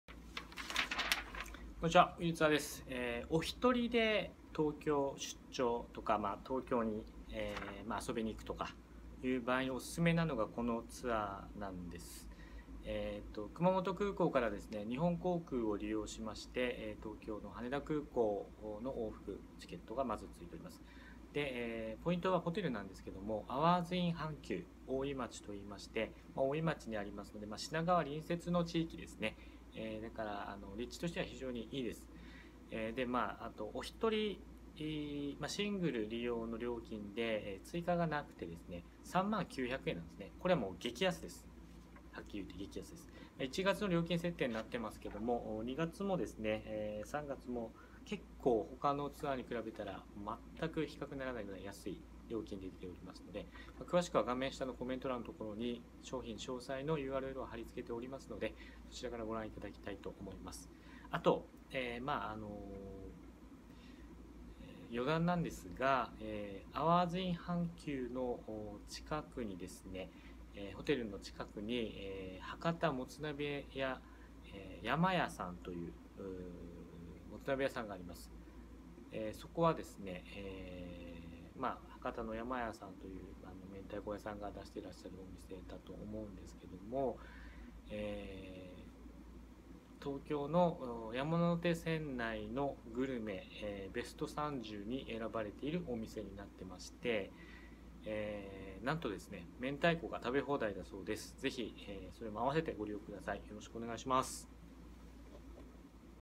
【１名参加】熊本発着≪激安≫ＪＡＬで行く東京フリープラン２日間・アワーズイン阪急｜ウィルツアー
熊本発着の日本航空（羽田便）を利用したプランでで驚くほどの低価格を実現しています。出発日限定で超・格安の東京フリープランです。【商品詳細】